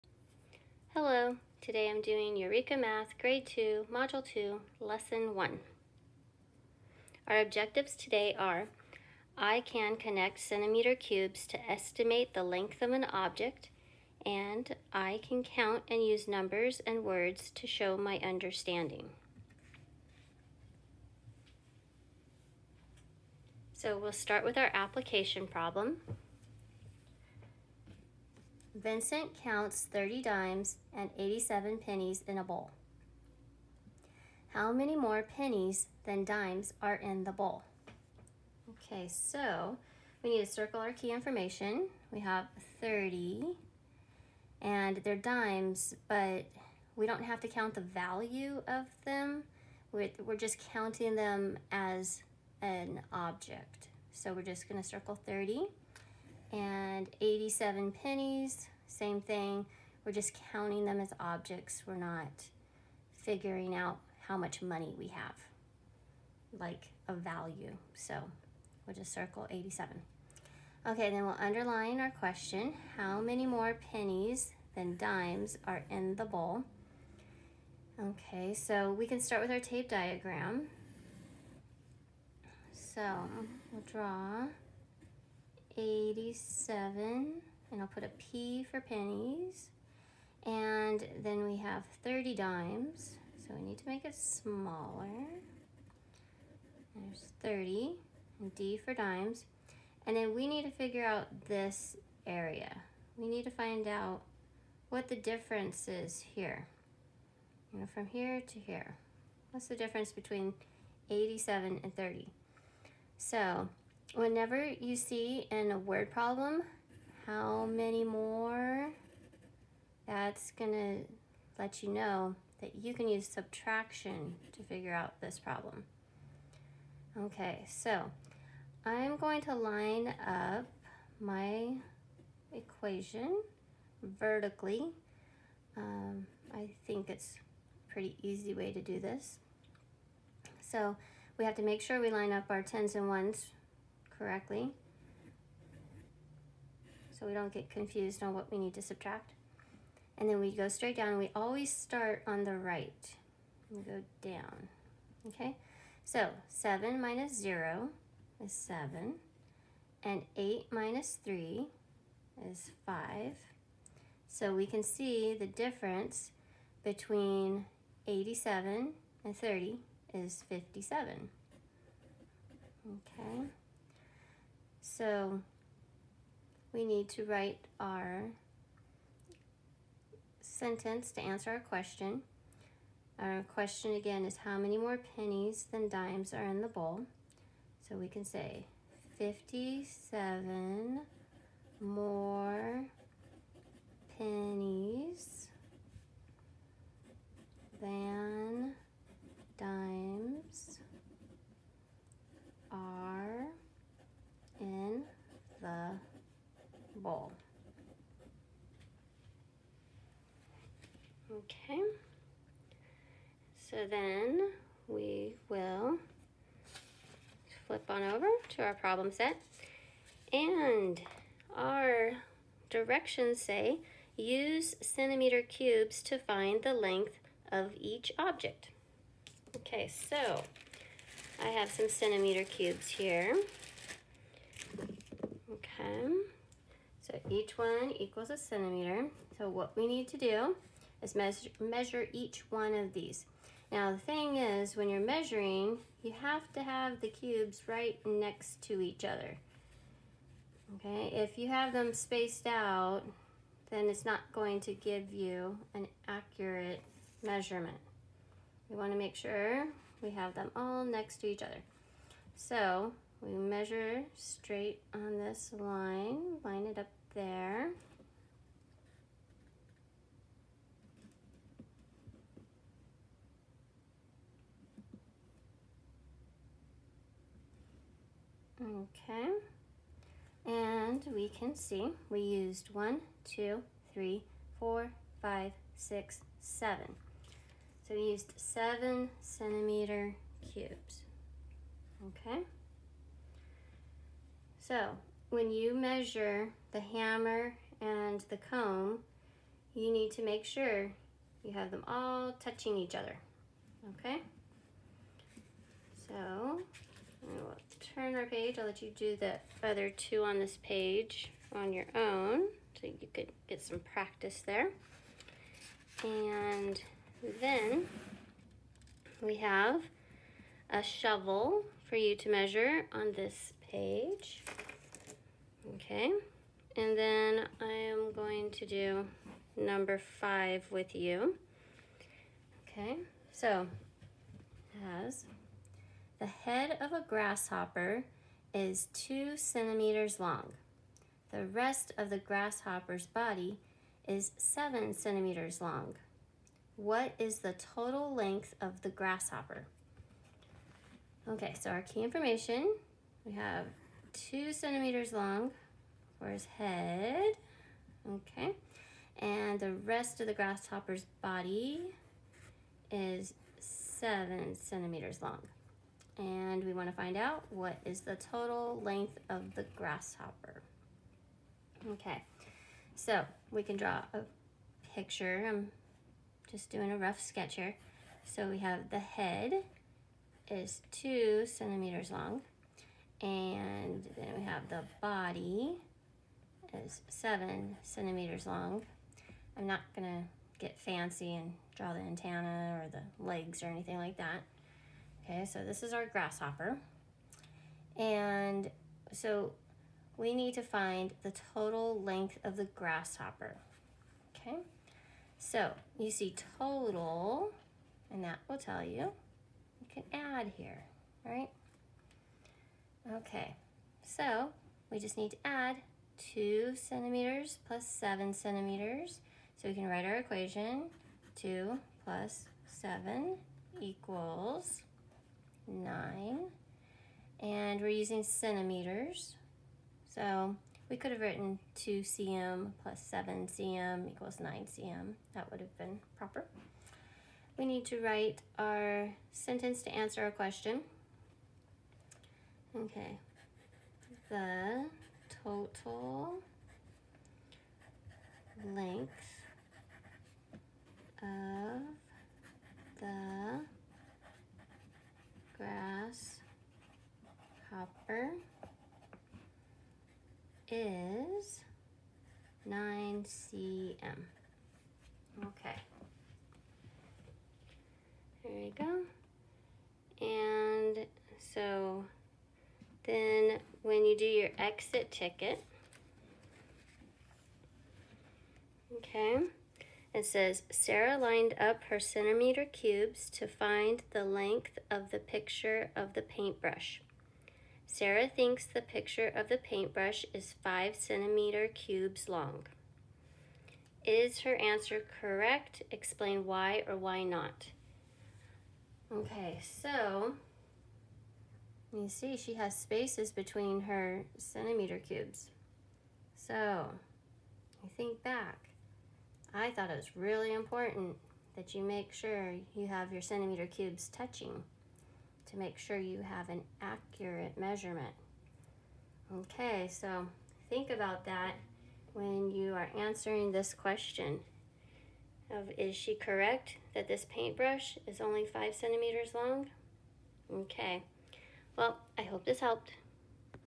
Eureka Math, Grade 2 [Module 2, Lesson 1]
Kid friendly objectives
Application Problem and some Problem Set problems worked through
 Centimeter Measurement using centimeter cubes